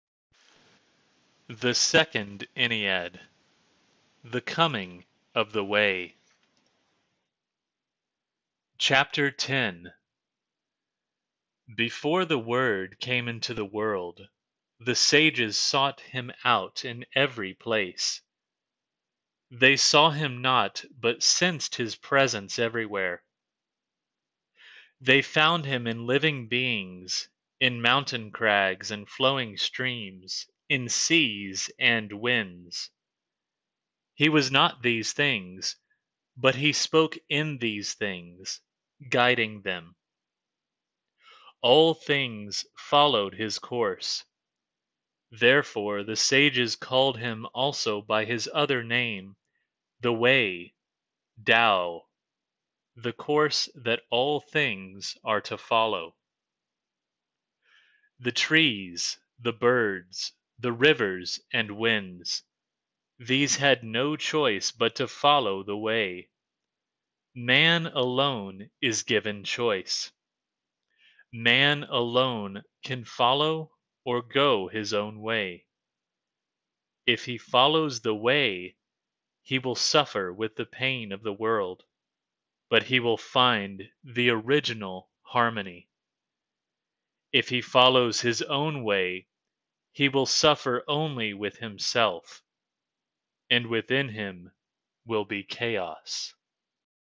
Christ the Eternal Tao: The Second Ennead, Chapter Ten
The Second Ennead/Chapter Ten from the book 'Christ the Eternal Tao' by Hieromonk Damascene.

Text:
Before the Word came into the world

The sages sought Him out in every place.

They saw Him not, but sensed His presence everywhere.

They found Him in living beings, in mountain crags and flowing streams, in seas and winds.

He was not these things,

But He spoke in these things, guiding them.

All things followed His Course.

Therefore the sages called Him also by His other name:

The Way (Tao),

The Course that all things are to follow.

The trees, the birds, the rivers and winds:

These had no choice but to follow the Way.

Man alone is given choice;

Man alone can follow or go his own way.

If he follows the Way, he will suffer with the pain of the world,

But He will find the Original Harmony.

If he follows his own way, he will suffer only with himself,

And within him will be chaos.